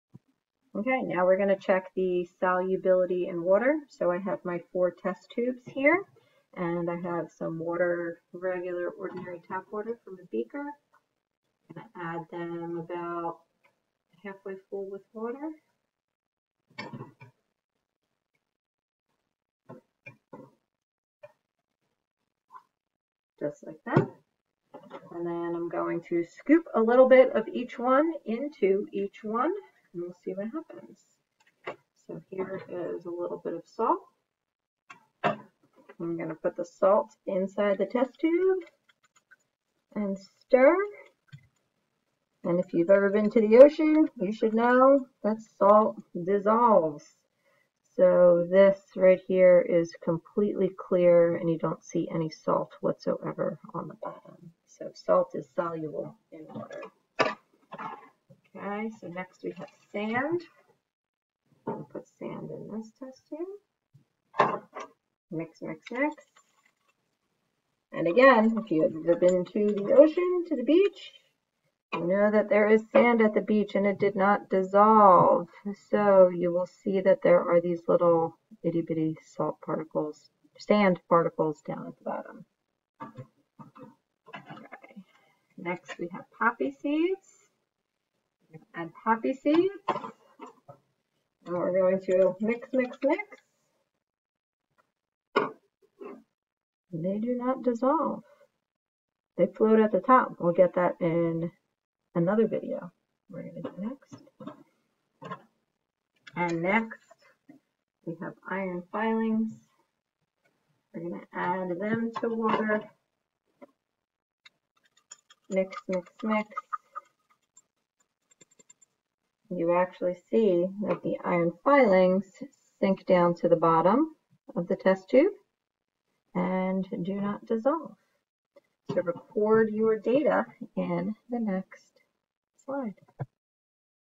Separation of a Mixture Lab - Solubility in Water
Make observations when each of the substances, salt, sand, poppy seeds, and water, are placed in a test tube with water to test for solubility.  Each substance is stirred with a stirring rod.